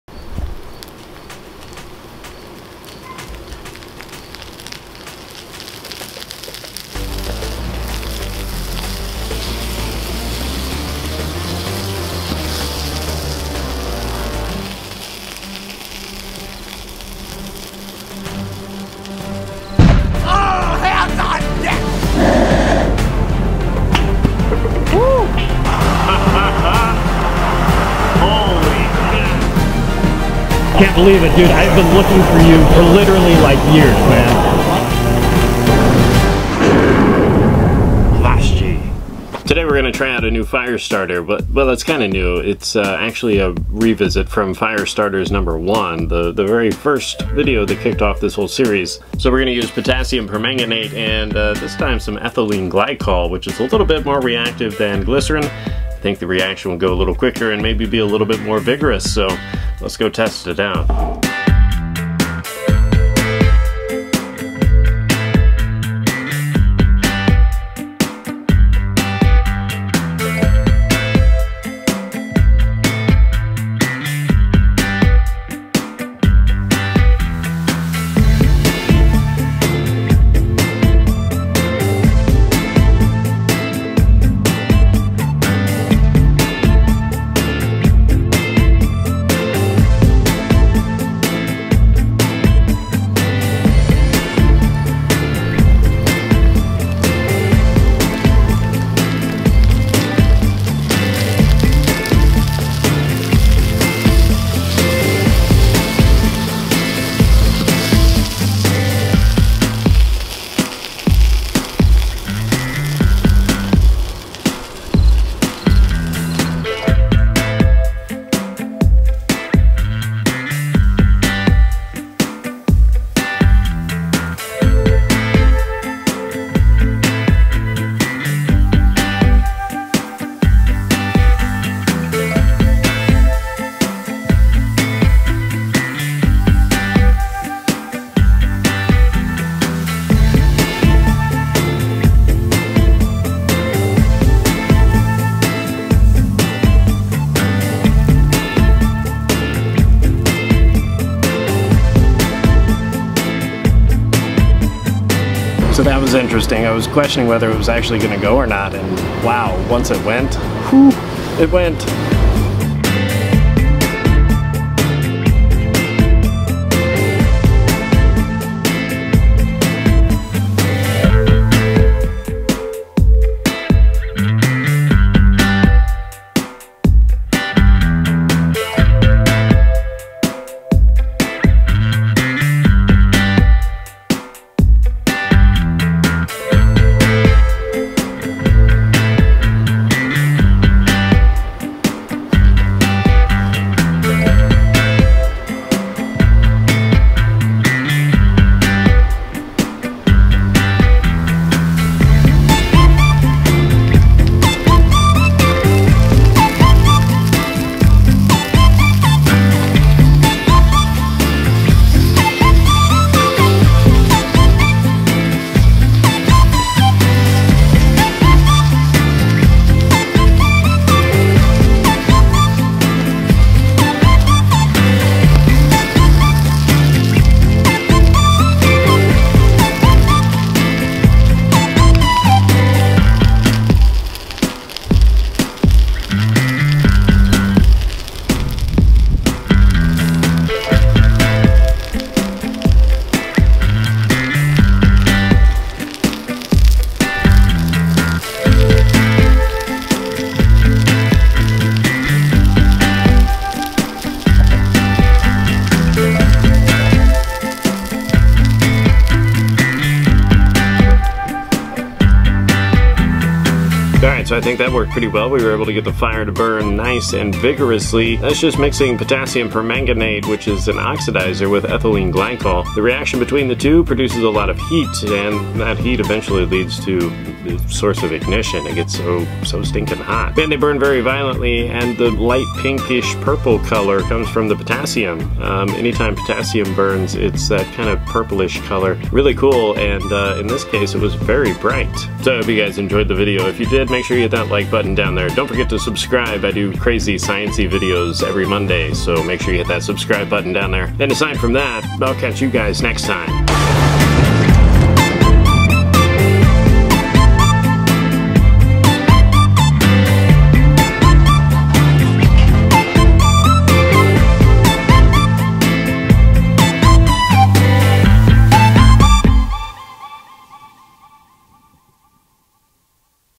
Fire Starters #10 - Permanganate & Ethylene Gylcol
What better way to celebrate TEN chemical fire starter videos than to improve upon the original fire starter!  In the very first video in the series, we used potassium permanganate and glycerin.  This time, we step it up by replacing the glycerin with ethylene glycol (ethanediol) to react with potassium permanganate to start a fire!  This chemical reaction is hot! Please DO NOT try this at home!!